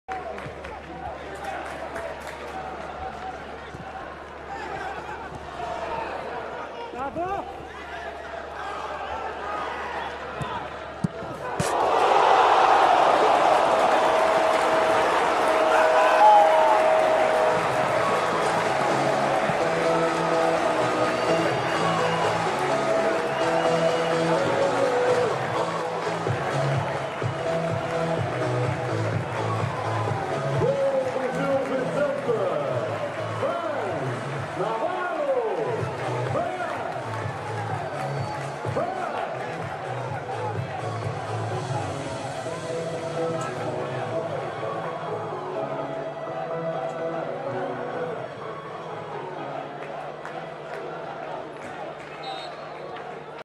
Goal | Golo F. Navarro: Gil Vicente (1)-1 FC Arouca (Liga 21/22 #11)
Zé Carlos na direita a conseguir passar por Abdoulaye, passe atrasado para o remate de Fran Navarro, a bola entrou junto ao segundo poste, sem hipótese para Victor Braga!